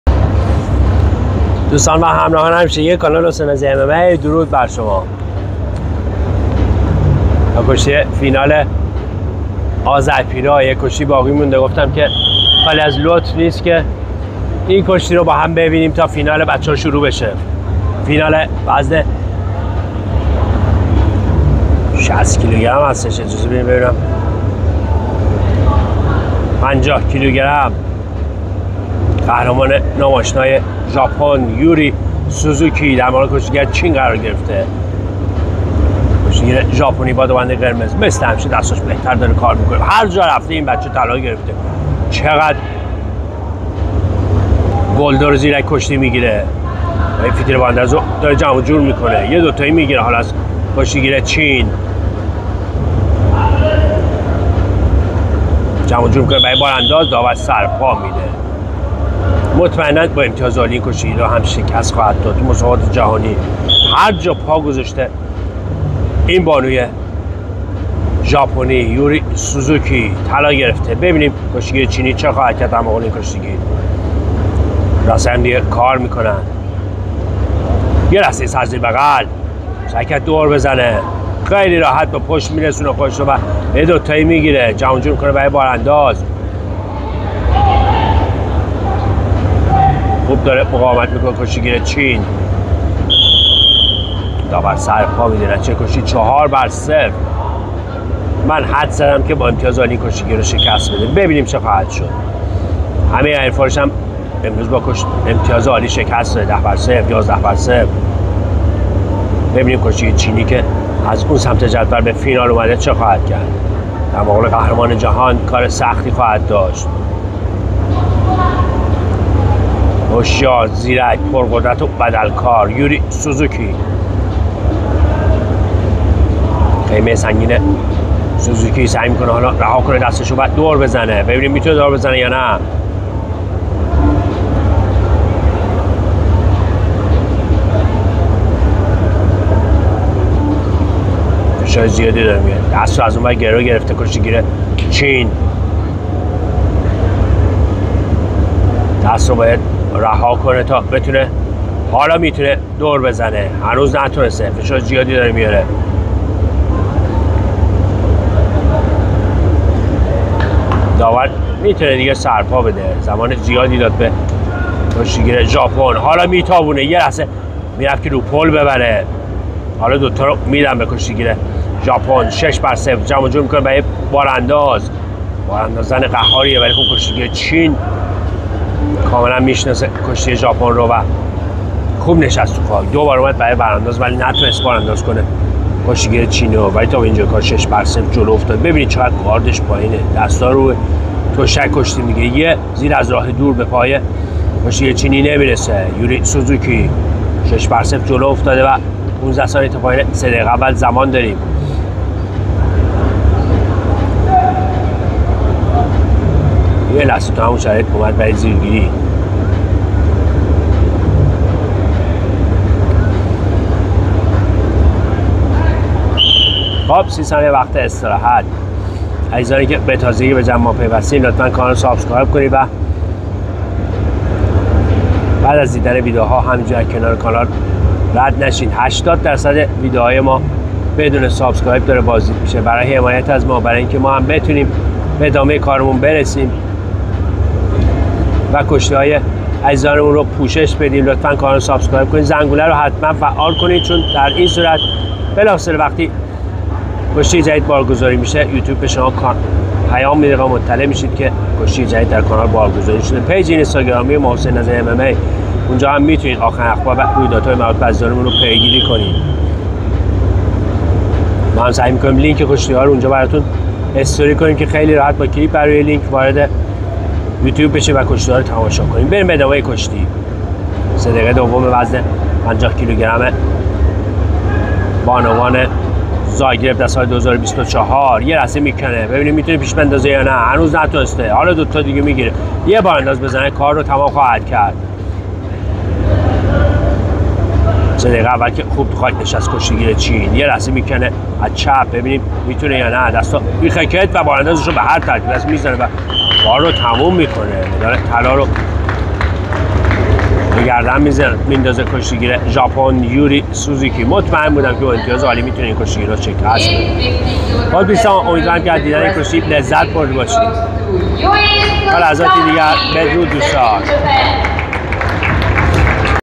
فینال بانوان زاگرب کرواسی سوزوکی[ ژاپن ]در مقابل چین50KG🤩🤼2024
The international freestyle and Greco Romes wrestling competitions of the Zagreb Open Cup and the first ranking of the World Wrestling Union in 2024 were held in Croatia from the 10th to the 15th of January, during which the Iranian national freestyle wrestling team took second place.  Iran's national team participated in wrestling with four wrestlers, who ended their career with one gold and one silver

رقابت‌های بین‌المللی کشتی آزاد و فرنگی  جام اوپن زاگرب و نخستین رنکینگ اتحادیه جهانی کشتی در سال ۲۰۲۴ روزهای دهم تا پانزدهم ژانویه  در کشور کرواسی برگزار شد که طی آن تیم ملی کشتی آزاد  ایران به مقام دوم رسید.    در کشتی فرنگی هم تیم ملی ایران با چهار کشتی گیر شرکت کرده بود که با یک طلا و یک نقره به کار خود پایان داد

                                                     INSTAGRAM ; HOSSEINNAZARIMMA
                                                                  REPORTER : ALI NAZARI